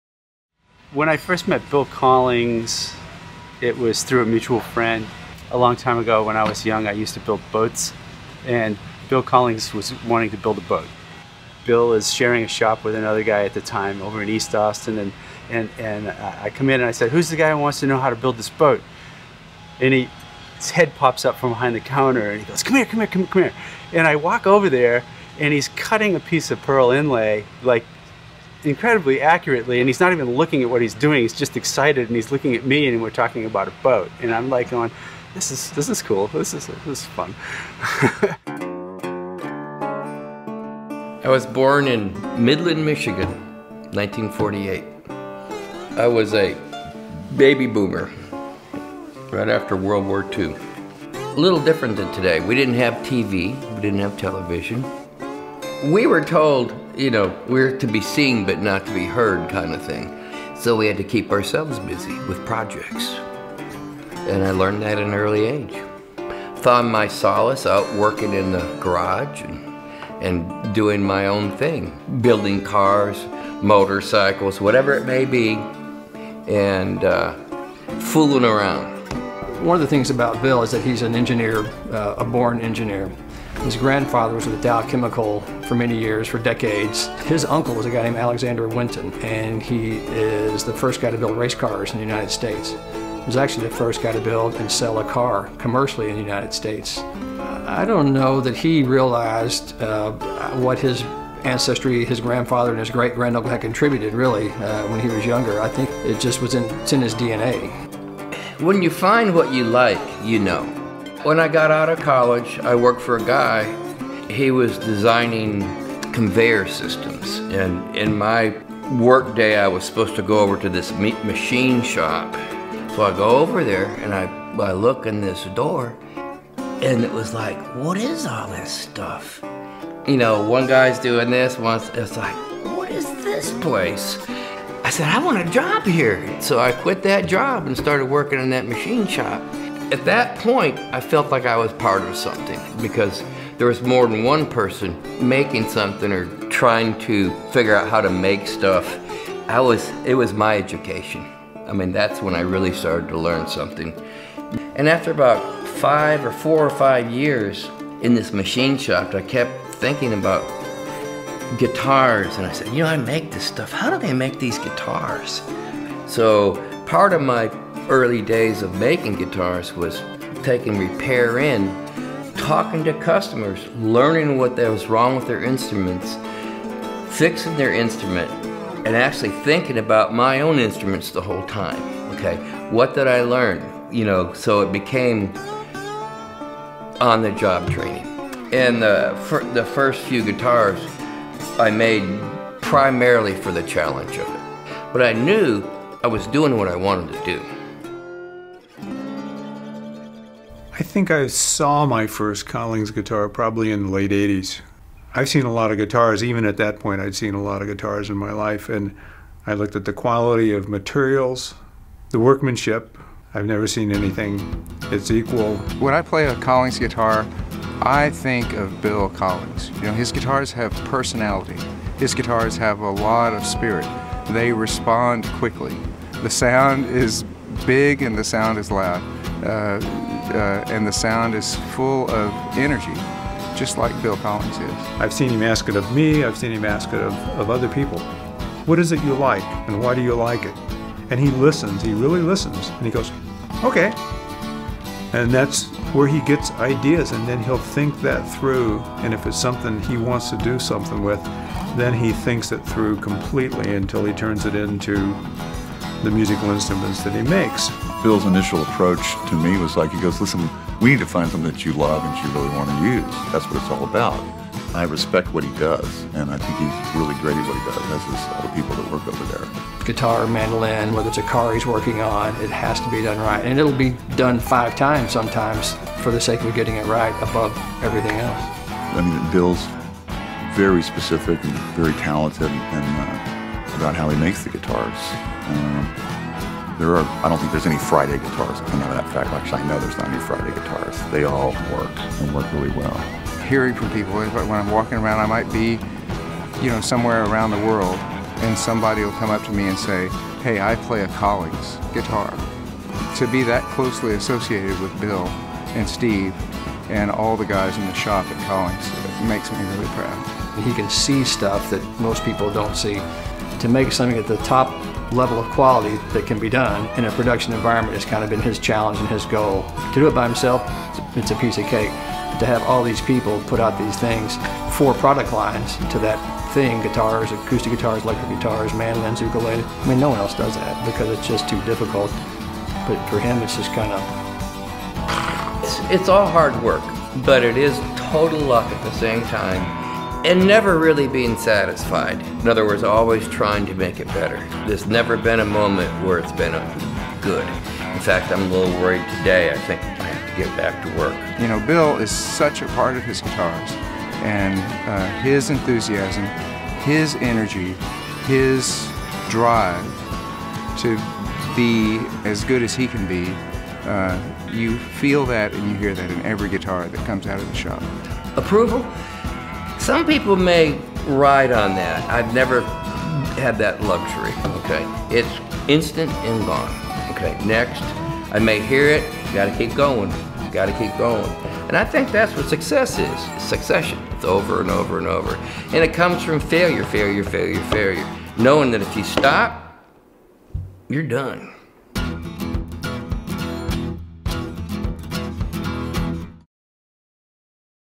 The Bill Collings Story - Collings Guitars
" 天才 "ビル・コリングスのヒストリーが語られております。